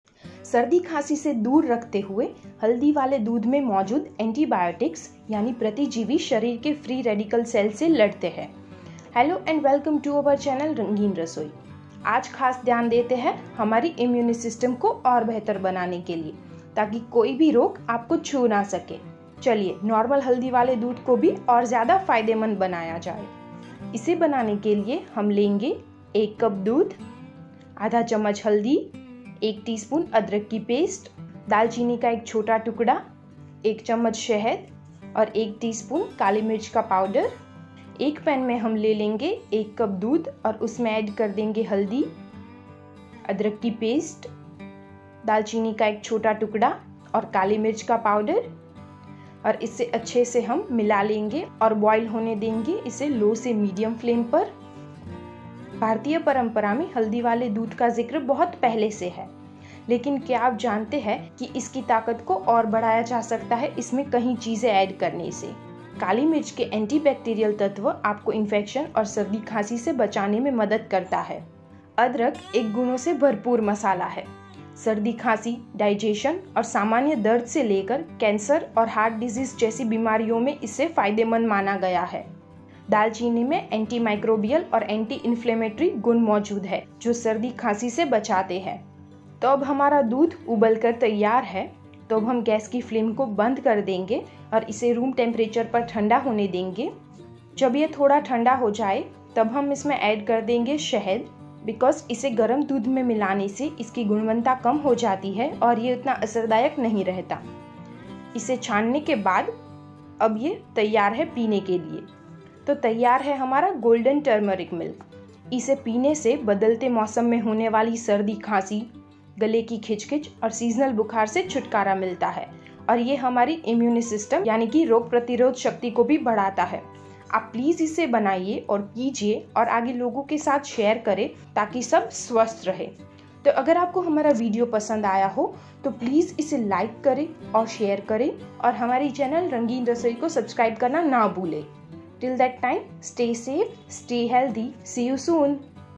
Immunity Power बढ़ाता है ये हल्दी वाला दूध Golden Turmeric Milk Boost Your Immunity
Immune Power बढ़ाता है ये हल्दी वाला दूध Golden Turmeric Milk Boost Your Immunity

Indian ayurveda has appreciated turmeric milk since ages. Adding some other Indian spices to it makes it more fruitful to boost your immunity power.

PLEASE DO SHARE IT AND HAVE IT AS IT IS OUR PRIOR NEED AND RESPONSIBILITY TO SAVE PEOPLE AROUND US.

Keep Supporting!!